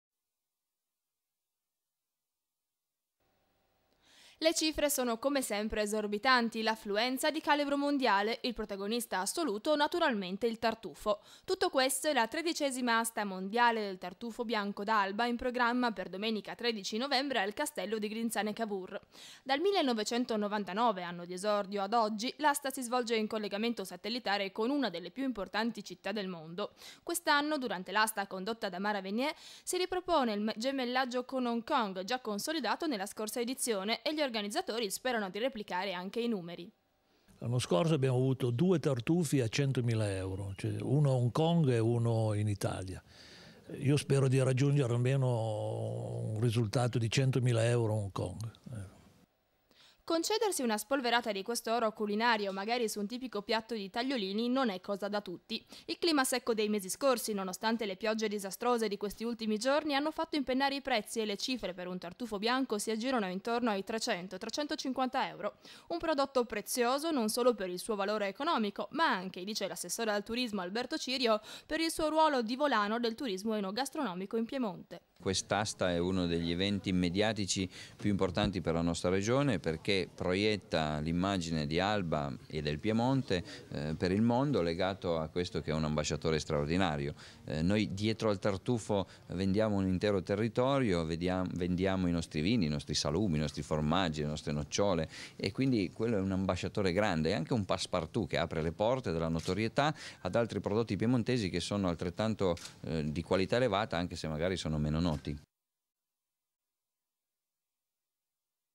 Fiera del Tartufo D'Alba
Alba torna sul palcoscenico mondiale con l'ormai tradizionale asta del tartufo bianco d'alba, appuntamento imperdibile per gli appassionati. Quest'anno l'asta sarà di nuovo in collegamento con Hong Kong.